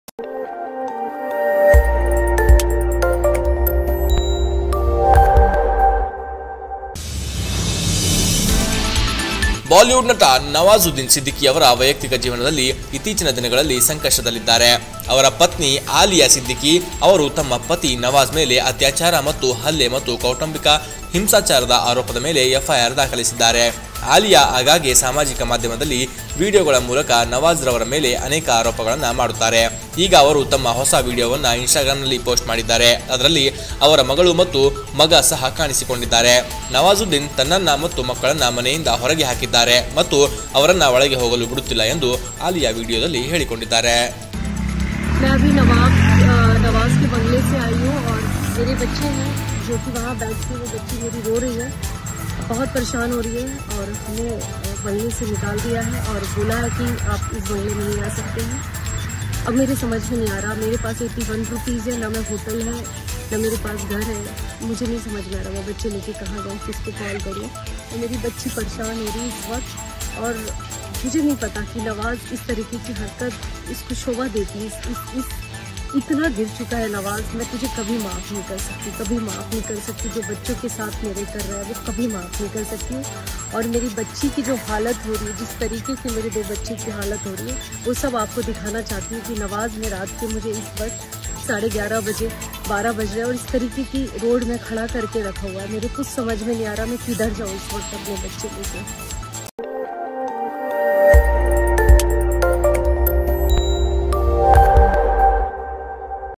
Nawazuddin Siddiqui : ಹೆಂಡತಿ - ಮಕ್ಕಳನ್ನು ಮಧ್ಯರಾತ್ರಿ ಮನೆಯಿಂದ ಹೊರಹಾಕಿದ ನಟ ಸಿದ್ದಿಕಿ.. ವಿಡಿಯೋ ವೈರಲ್!! KK
Nawazuddin Siddiqui : ಹೆಂಡತಿ - ಮಕ್ಕಳನ್ನು ಮಧ್ಯರಾತ್ರಿ ಮನೆಯಿಂದ ಹೊರಹಾಕಿದ ನಟ ಸಿದ್ದಿಕಿ.. ವಿಡಿಯೋ ವೈರಲ್!! KK NEWS KANNADA